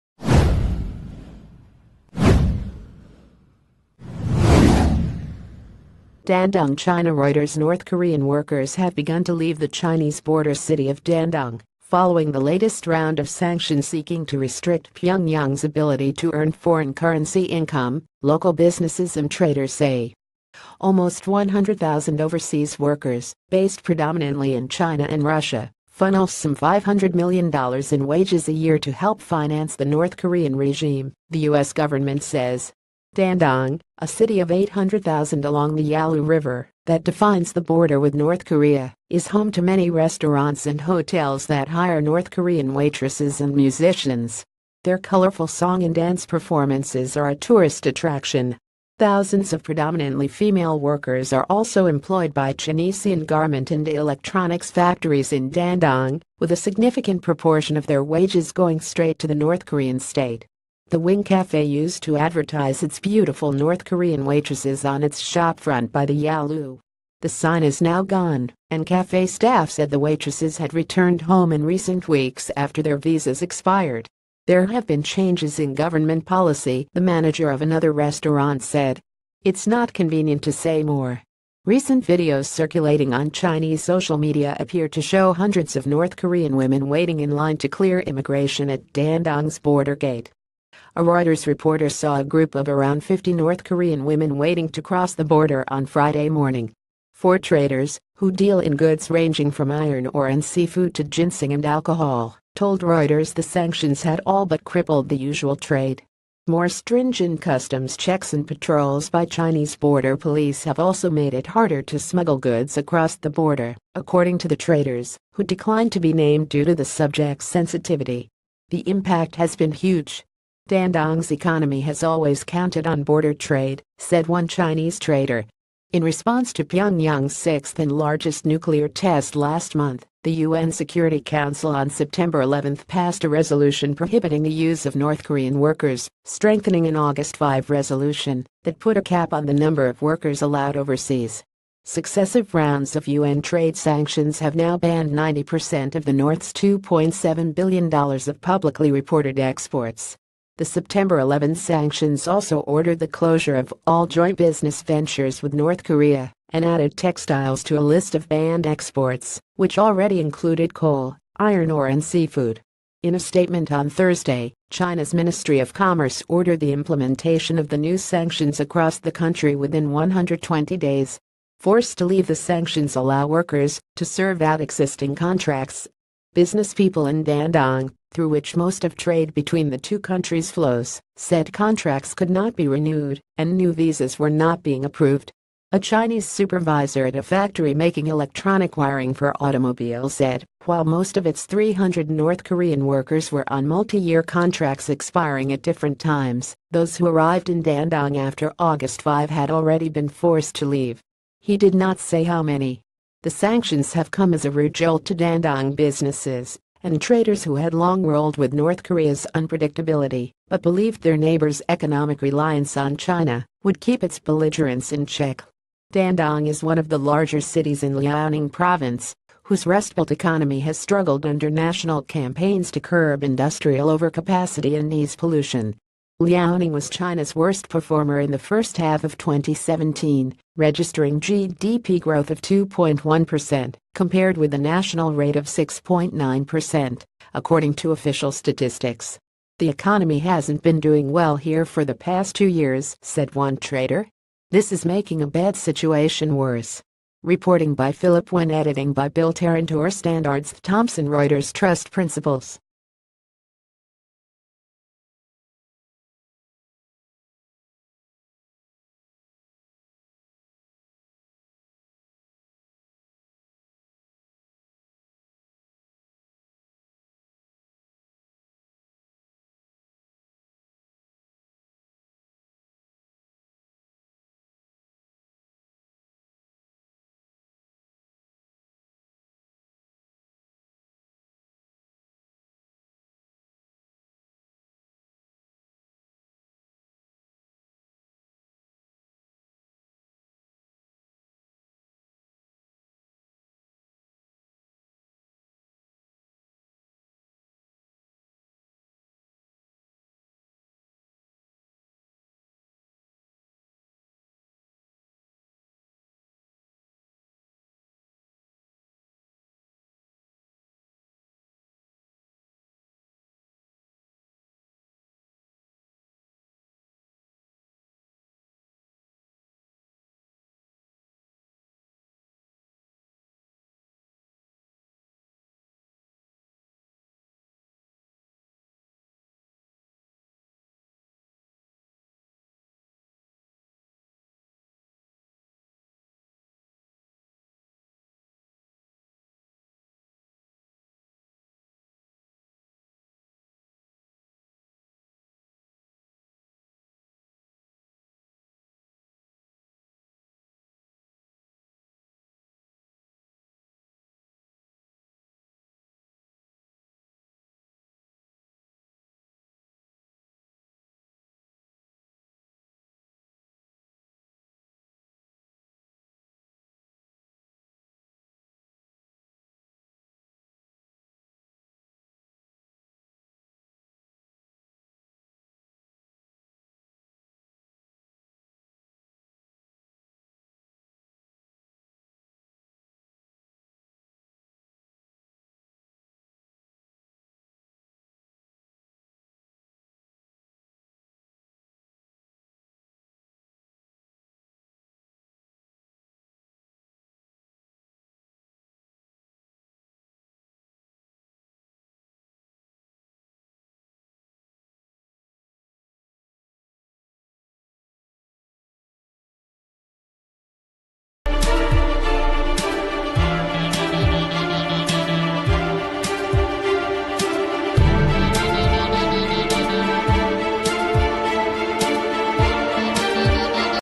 Breaking News | As sanctions bite, north korean workers leave chinese border hub
As sanctions bite, north korean workers leave chinese border hub

DANDONG, China Reuters North Korean workers have begun to leave the Chinese border city of Dandong, ...